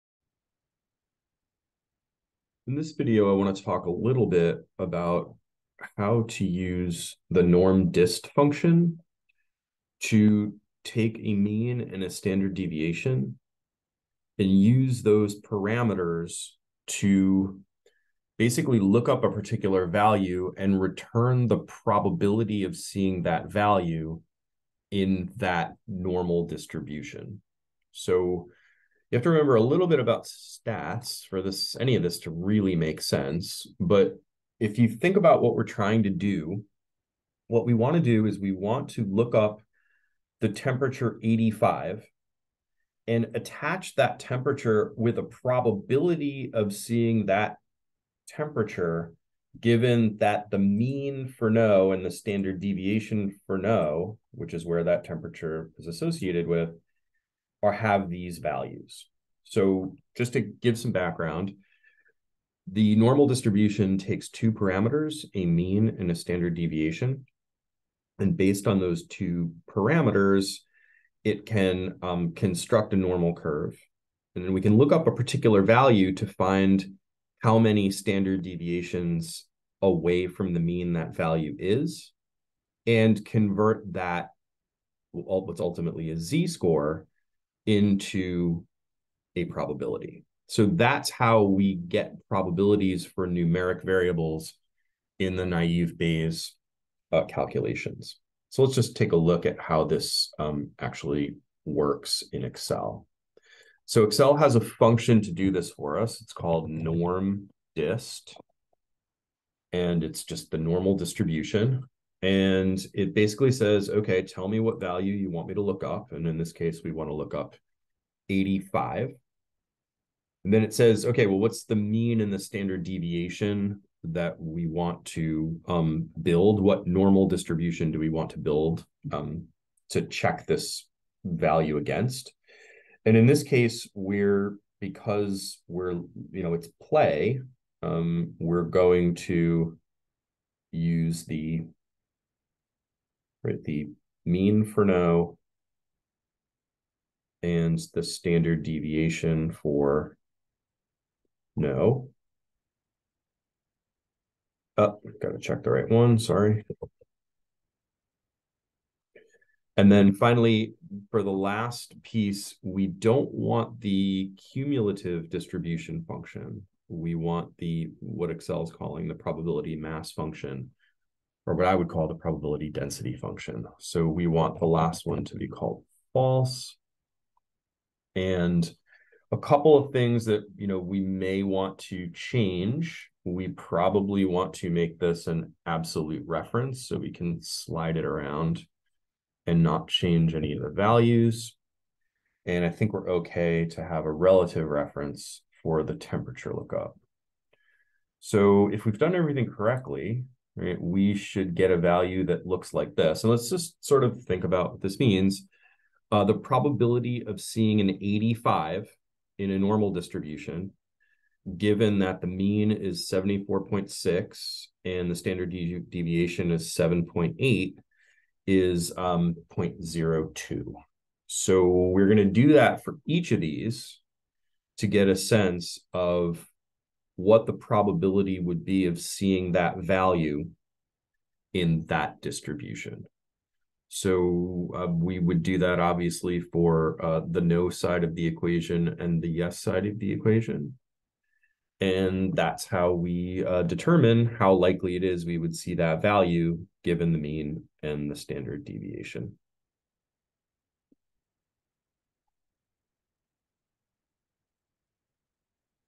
norm.dist() function in Excel
This video briefly explains how to use the norm.dist() function in Excel.